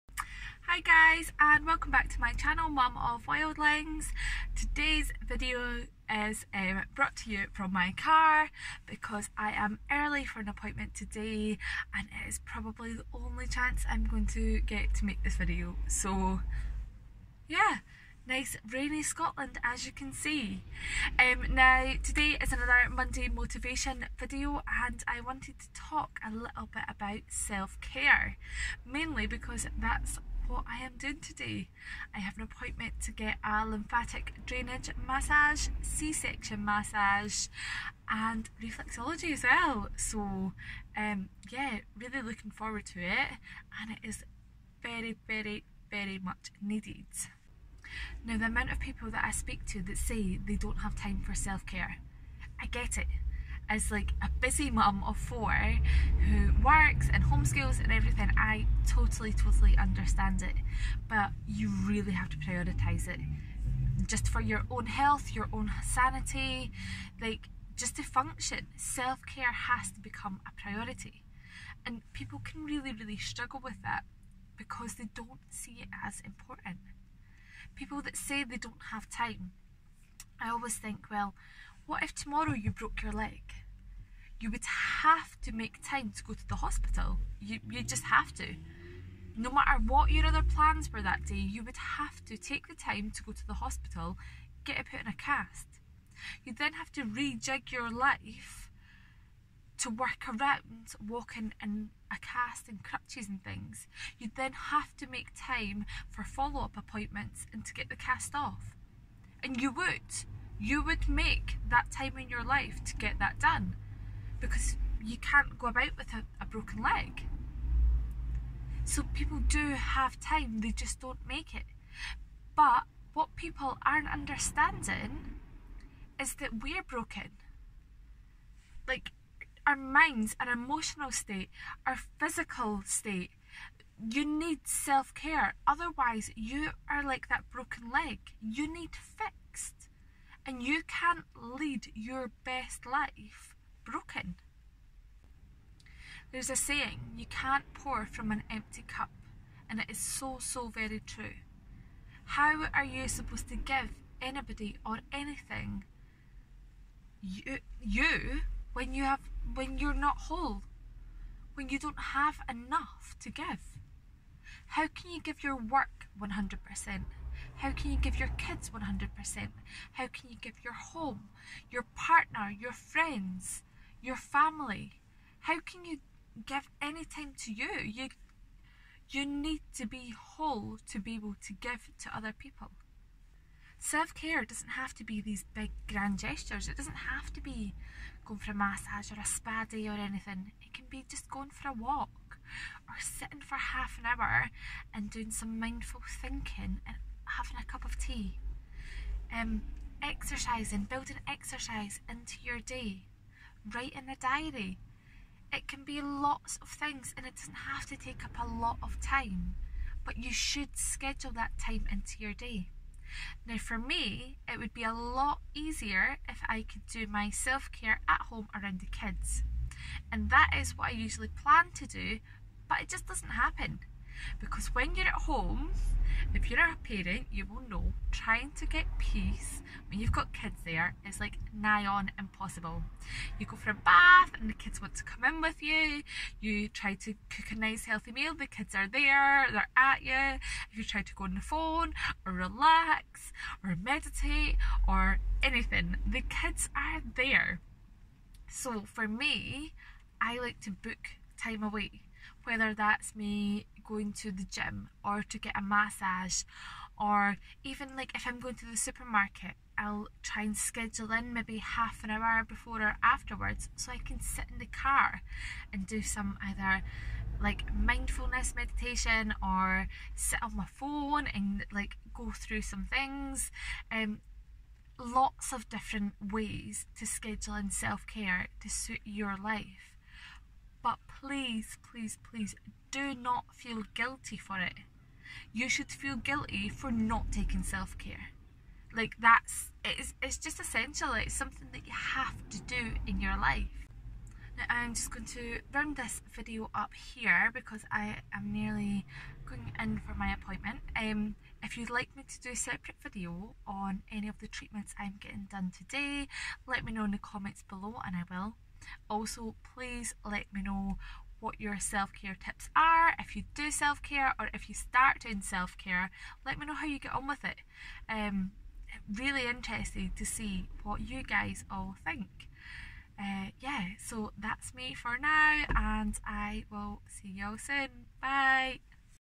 Self Care | Mum Vlog
Self care is so important for everyone but even more so when you are a parent that has other people dependent on you. We need to stop seeing it as a luxury and start seeing it as a priority.